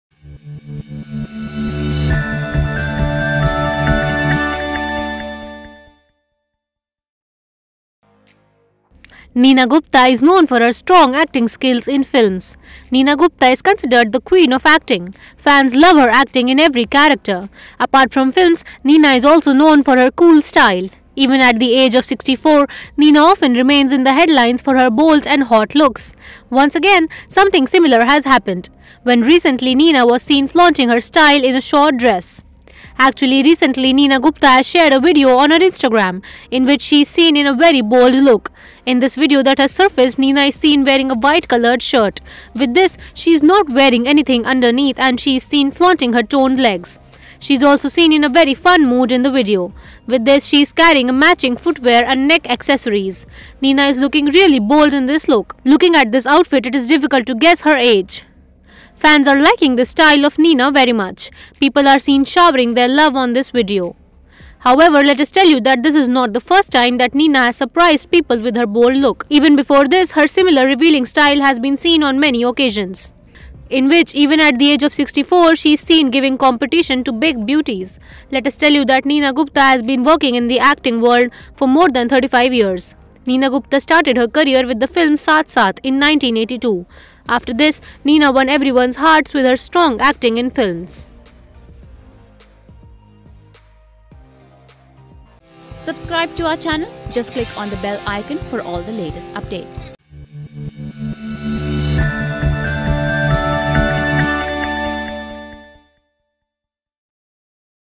At the age of 64, Neena Gupta shared a video wearing a short dress
Neena Gupta has given life to many wonderful films in her 35 years of career. Everyone is crazy about his strong acting. Apart from acting in films, Neena Gupta also remains in the news for her glamorous style. Something similar happened this time too when she recently shared a video of herself in a short dress.